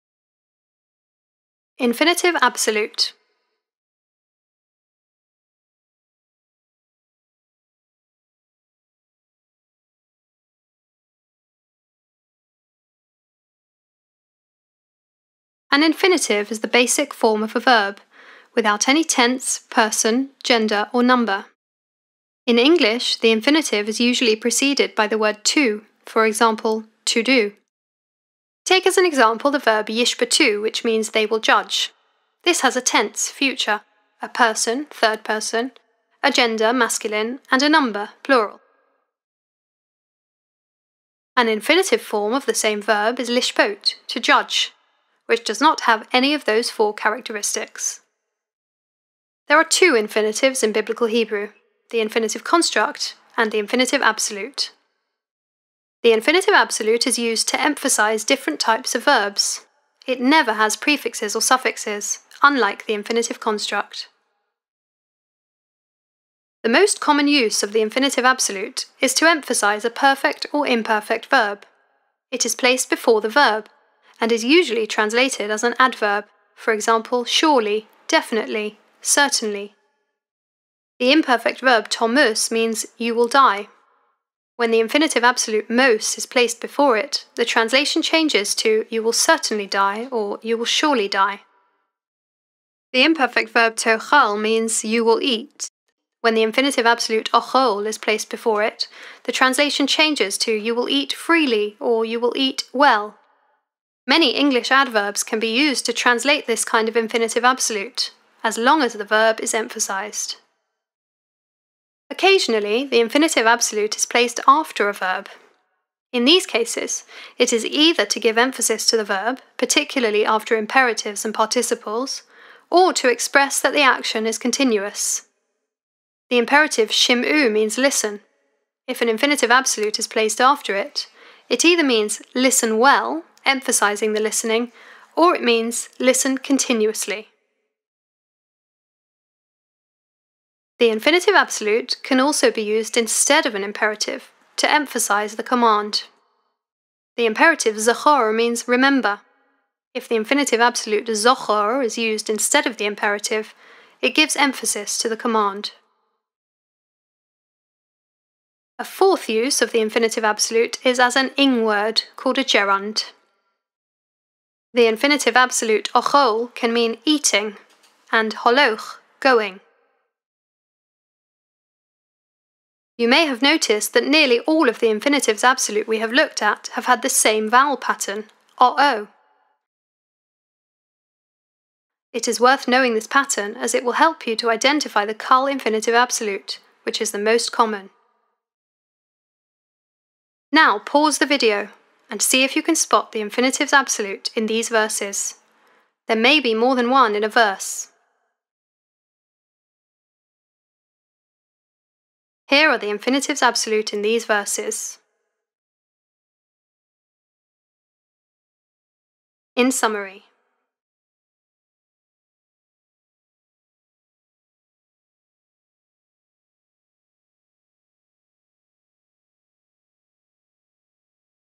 Infinitive Absolute
Learn about the infinitive absolute in Biblical Hebrew.

Copyright M.Goldblatt 2018; design by Velorose